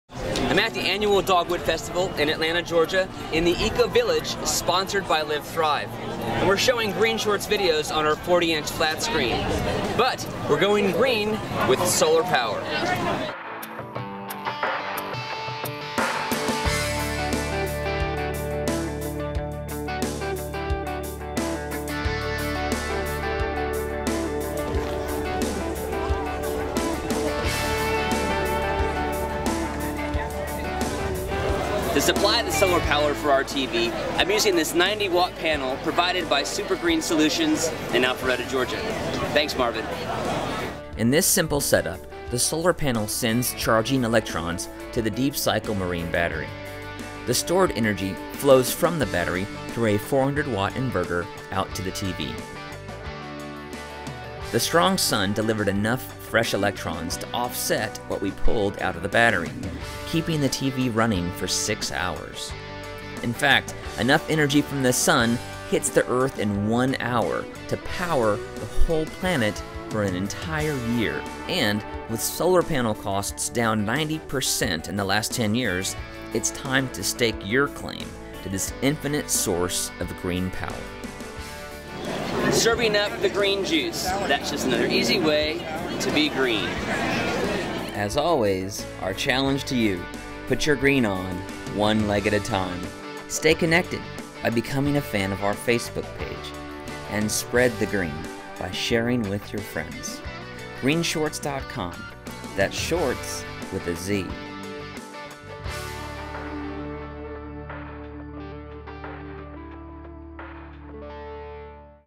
How To Power A TV With Solar Power
Green Energy in Georgia - How to power a TV with Solar Power At the Annual Dogwood Festival in Atlanta Georgia, in the EcoVillage, Tom Mills powered a 40” Flat Screen TV with Solar Power using a 90 Watt solar panel provided by Super Green Solutions in Alpharetta Ga.